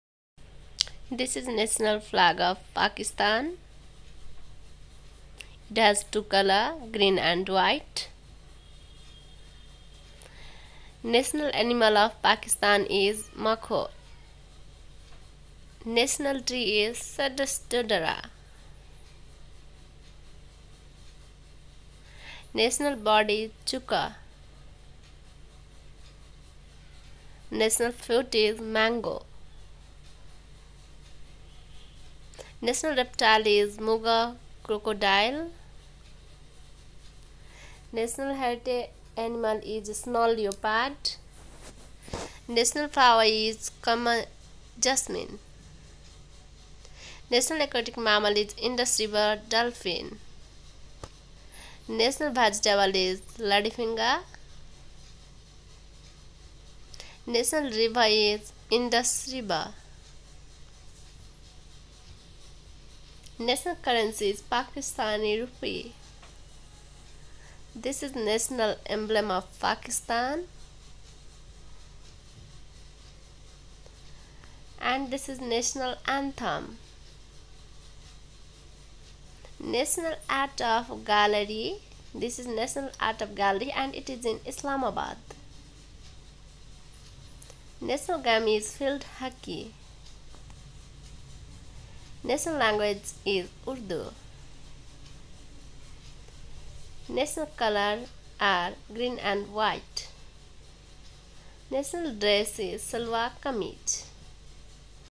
National symbols of Pakistan/ Pakistan's National symbols पाकिस्तान के राष्ट्रीय चिह्न क्या क्या है?
In this video you will find all the national symbols of Pakistan. Here are all National symbols of Pakistan with images. If you are visiting Pakistan then you must know all the national emblem of Pakistan. All the students should know about the national symbols of Pakistan also. The education should be in huge quantities to students so you can get all the knowledge about national symbols of Pakistan government in my video.